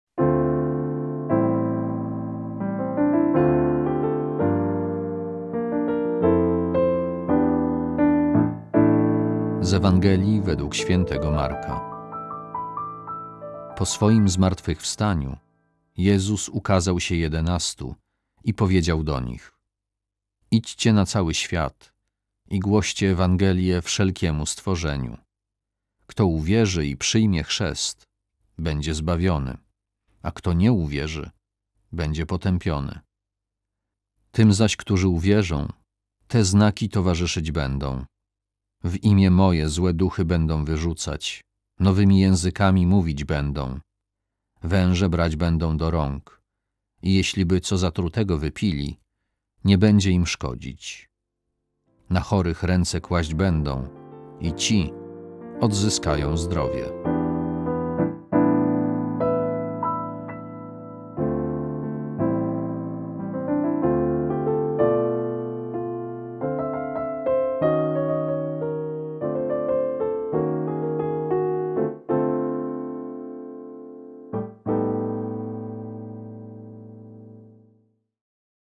Ewangelia | 25 stycznia 2020
Po swoim zmartwychwstaniu Jezus ukazał się Jedenastu i powiedział do nich:
„Idźcie na cały świat i głoście Ewangelię wszelkiemu stworzeniu. Kto uwierzy i przyjmie chrzest, będzie zbawiony; a kto nie uwierzy, będzie potępiony.
Tym zaś, którzy uwierzą, te znaki towarzyszyć będą: W imię moje złe duchy będą wyrzucać, nowymi językami mówić będą; węże brać będą do rąk, i jeśliby co zatrutego wypili, nie będzie im szkodzić. Na chorych ręce kłaść będą i ci odzyskają zdrowie”.